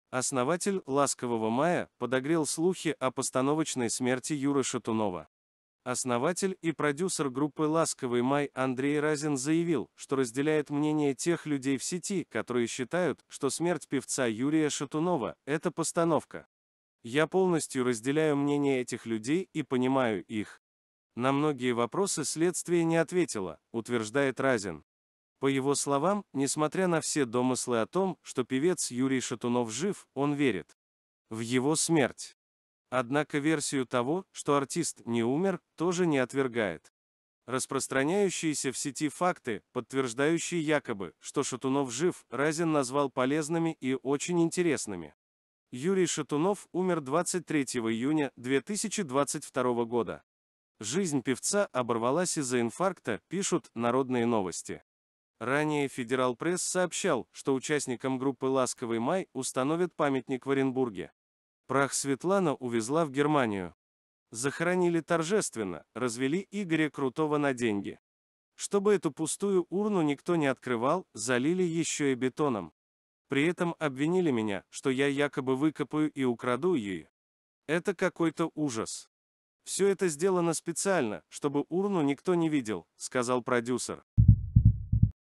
Разин сообщил о постановочной смерти Шатунова
«Прах Светлана увезла в Германию. Захоронили торжественно, развели Игоря Крутого на деньги. Чтобы эту пустую урну никто не открывал, залили еще и бетоном. При этом обвинили меня, что я якобы выкопаю и украду ее. Это какой-то ужас! Все это сделано специально, чтобы урну никто не видел», — сказал продюсер.